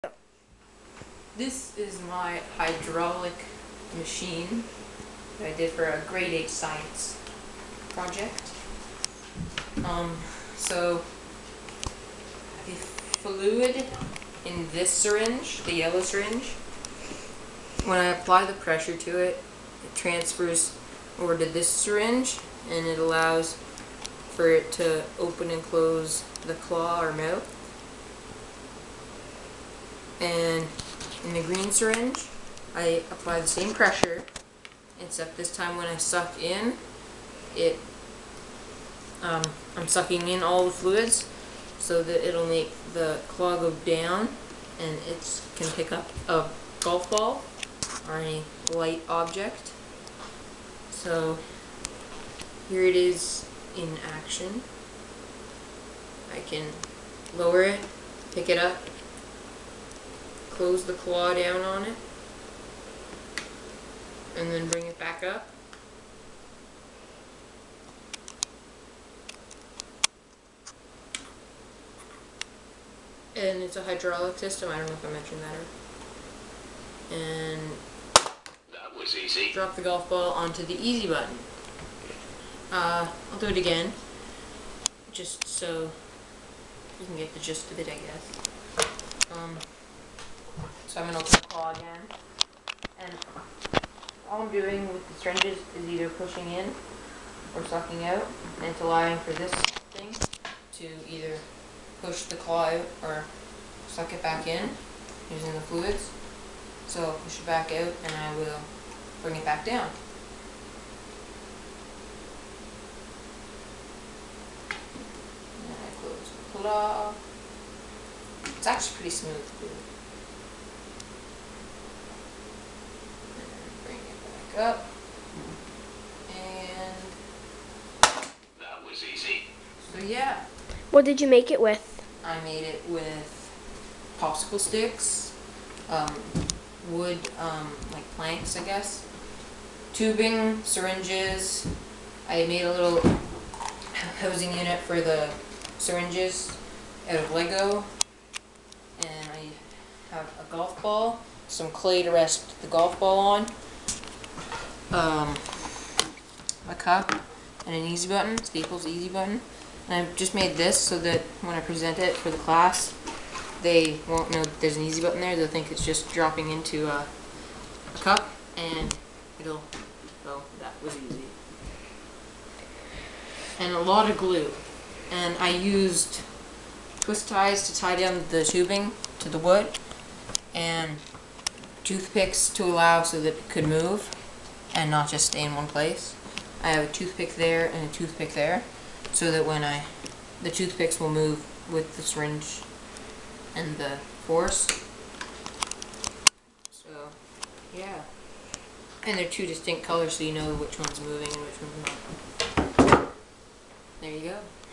Andrew's Grade 8 Science Project Hydraulic Arm with Syringes
Andrew's Grade 8 Science Project
Hydraulic Arm with Syringes